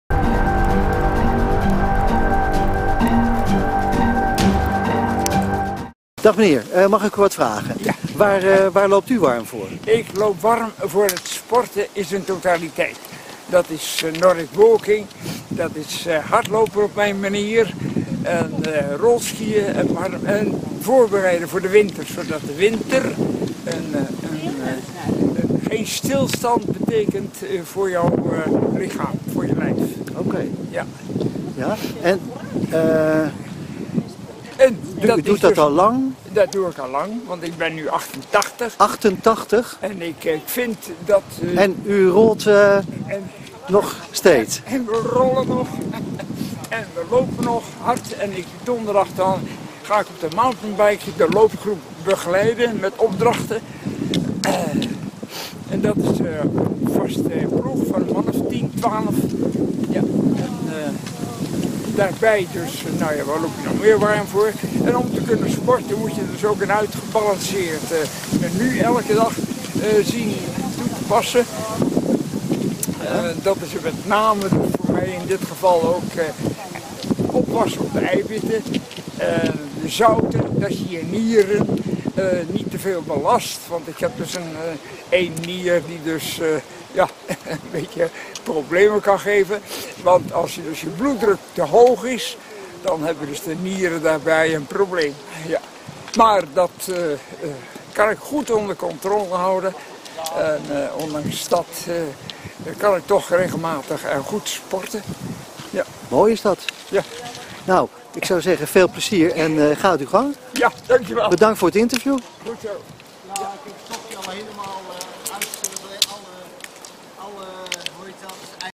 Mathieu
langlaufer van bijna 90 jaar, hoe blijf je fit.